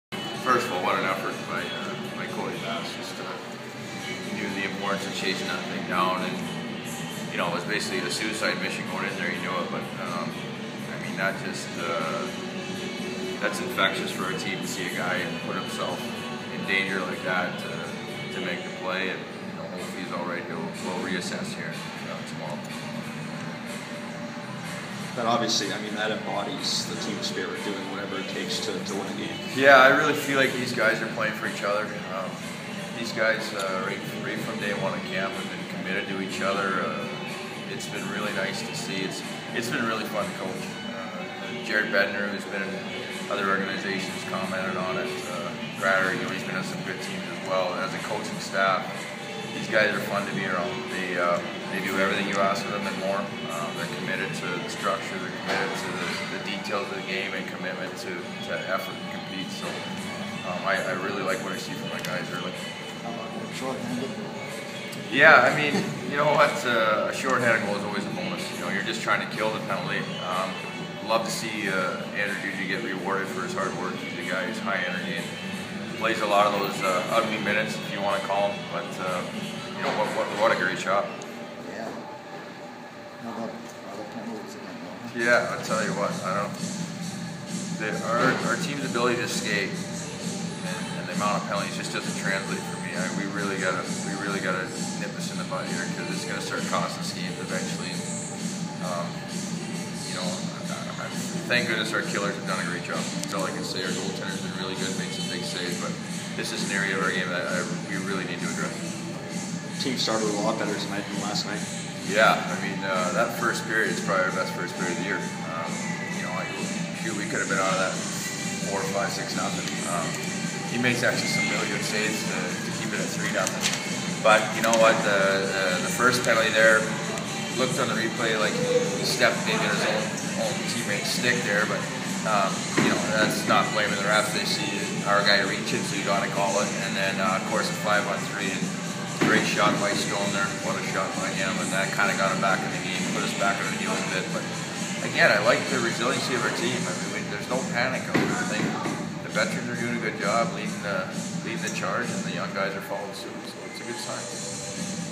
Falcons Postgame with Brad Larsen, Oct. 28
Falcons head coach Brad Larsen speaks following Springfield's 3-2 win over Portland on Sunday, Oct. 28 at the MassMutual Center.